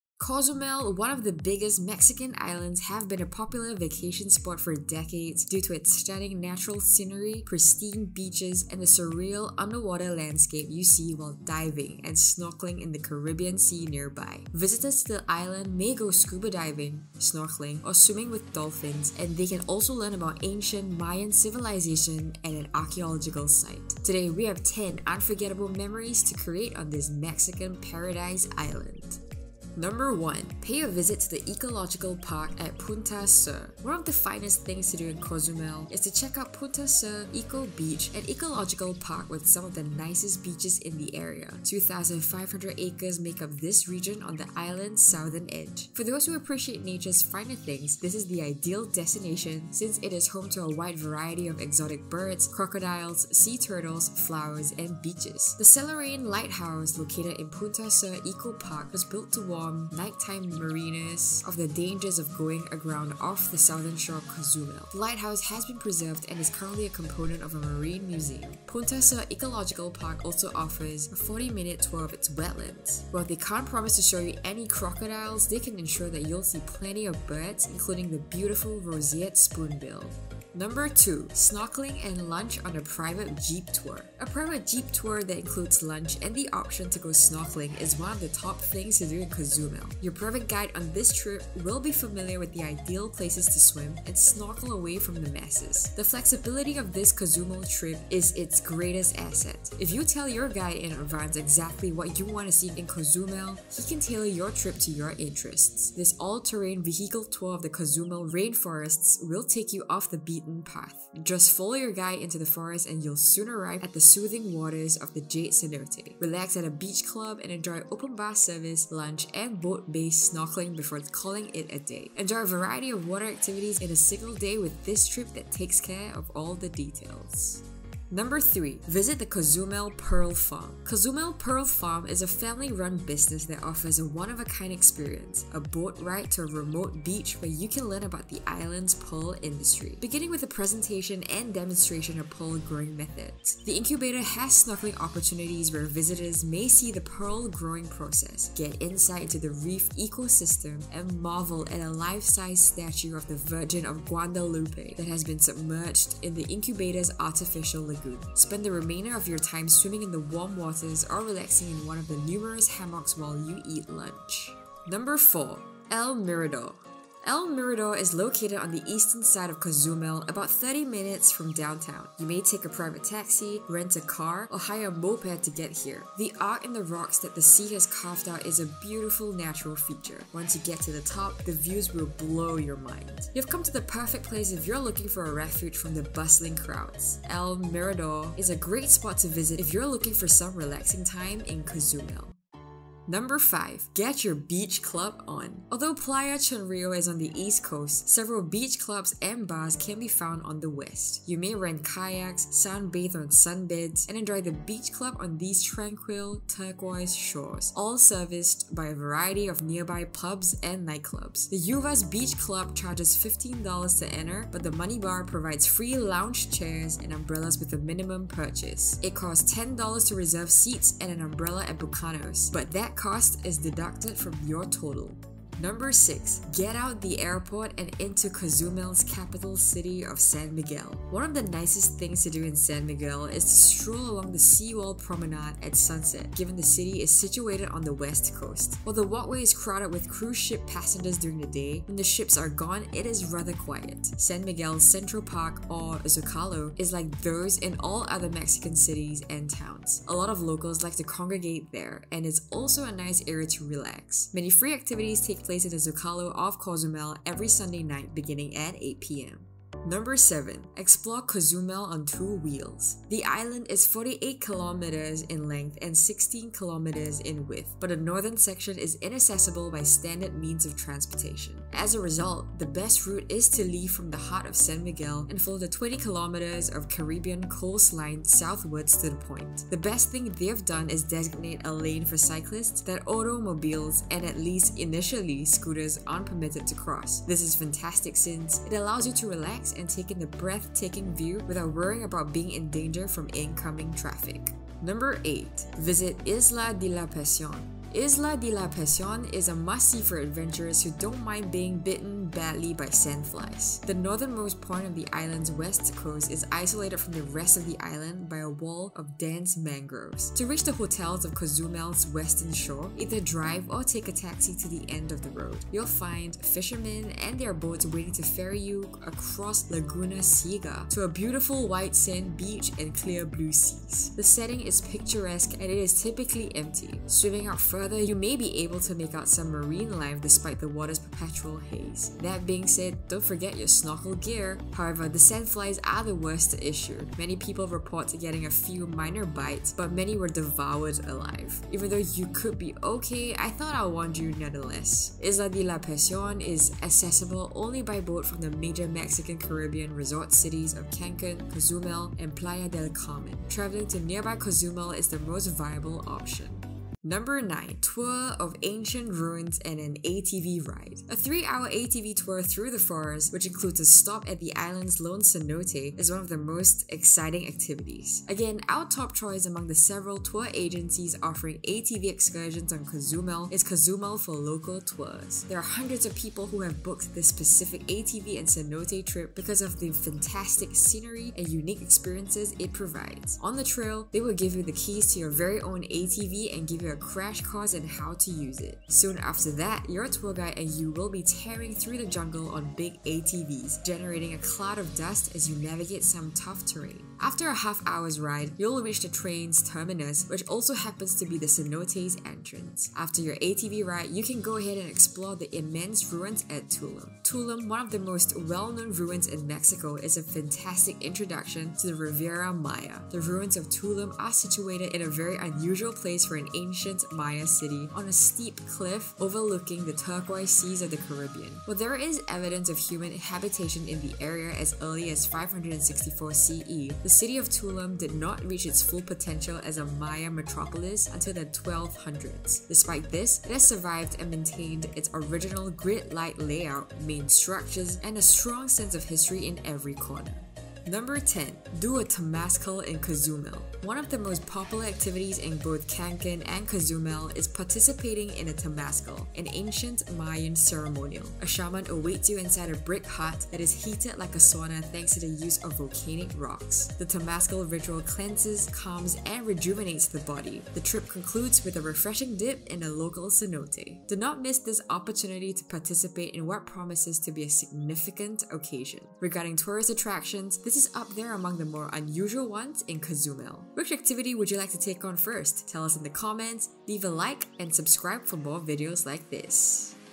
Top 10 Things to do in Cozumel Mexico - 2023 Travel Guide
Cozumel, one of the biggest Mexican islands, has been a popular vacation spot for decades due to its stunning natural scenery, pristine beaches, and the surreal underwater landscape you see while diving and snorkeling in the Caribbean Sea nearby.

Visitors to the island may go scuba diving, snorkeling, or swimming with dolphins, and they can also learn about ancient Mayan civilization at an archaeological site.

Today, we have ten unforgettable memories to create on this Mexican paradise island.